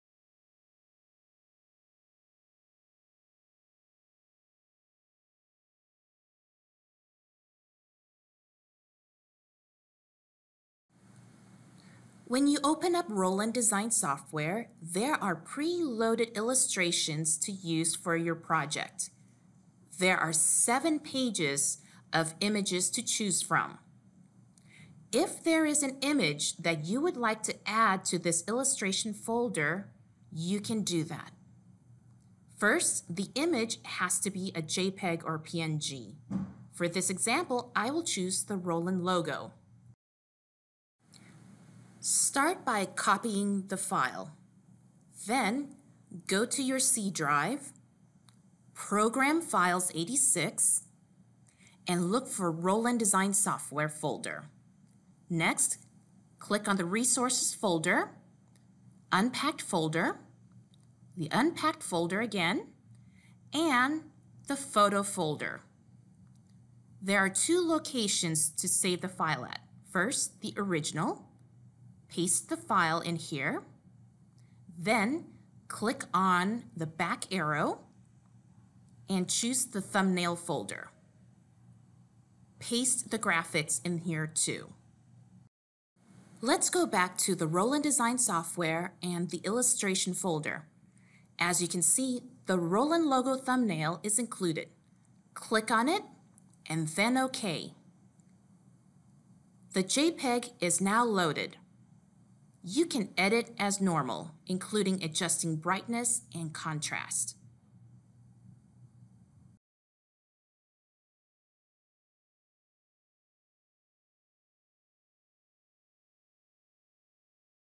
VersaStudio BT-12 - How to Add Images to Illustration Folder
This video shows how to add additional graphics to the pre-loaded Illustration library in Roland Design Software.